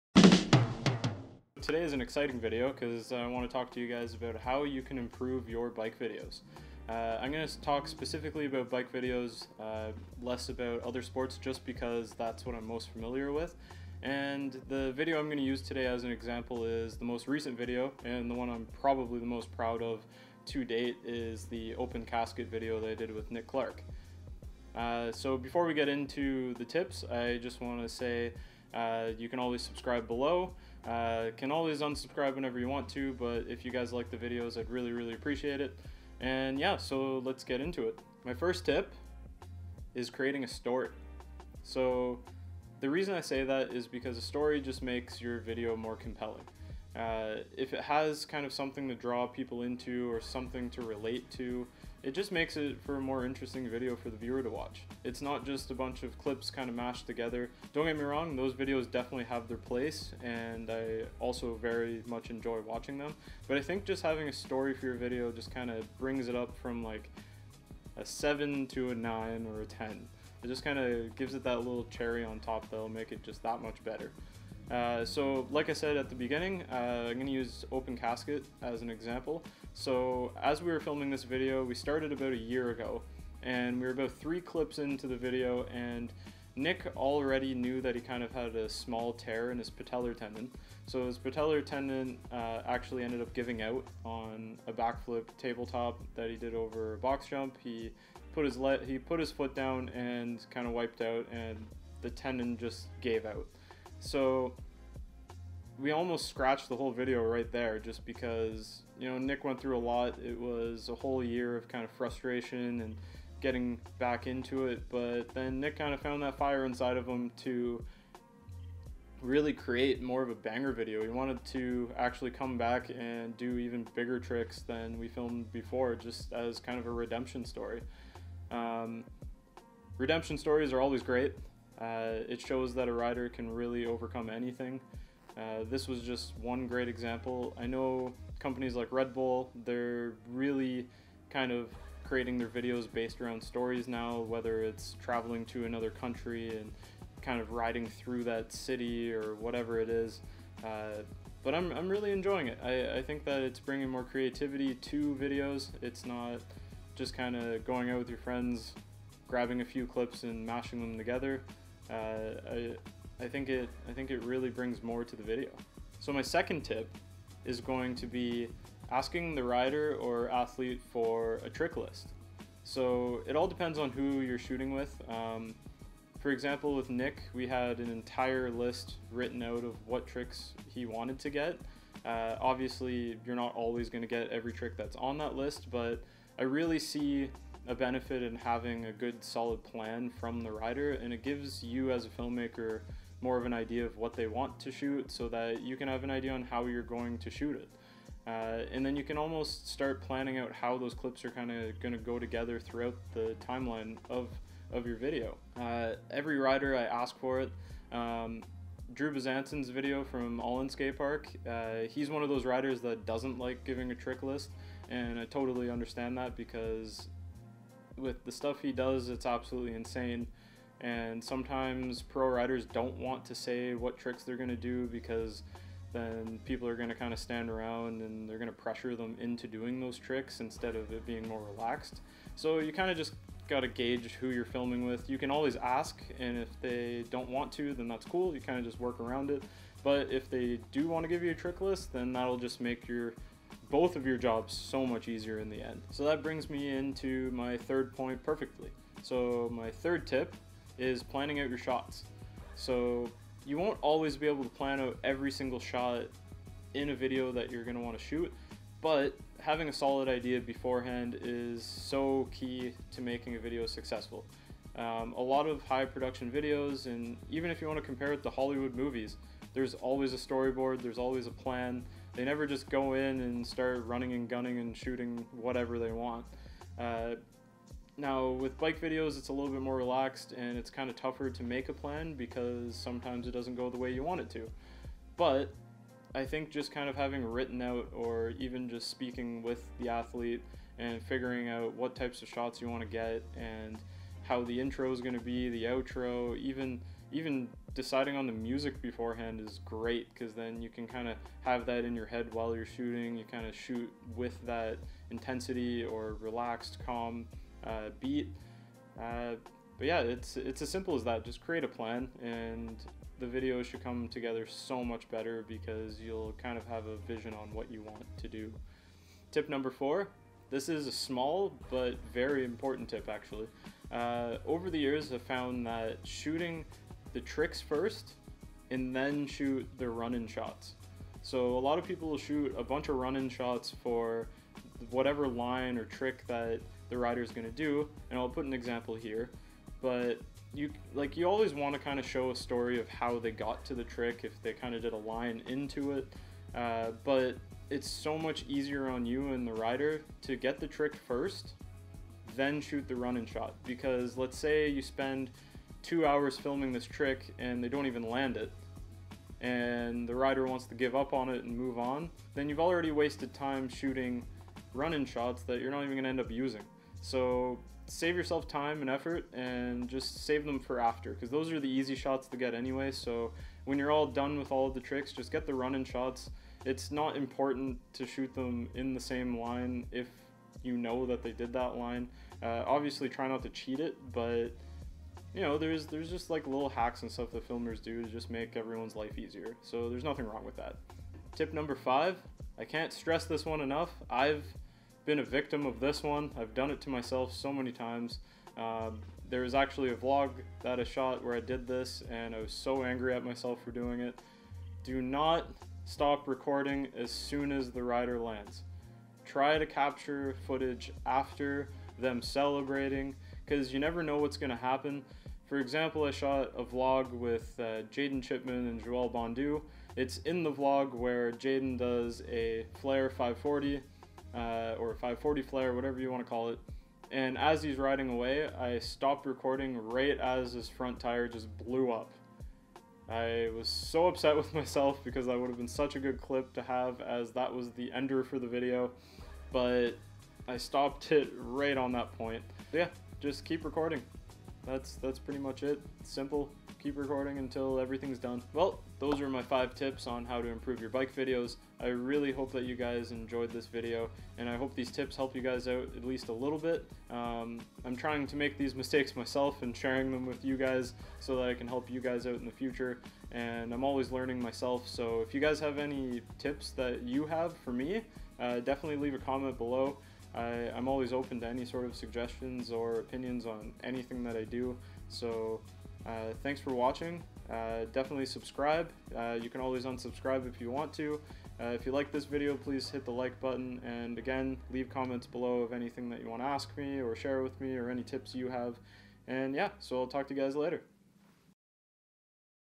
FRIDAY FIVE: HOW TO IMPROVE YOUR BMX/MTB VIDEOS
Starting a new segment called "Friday Five". This will be a collection of 5 tips every Friday for filmmakers and photographers alike. I hope these 5 quick tips help with any bike video you create in the future!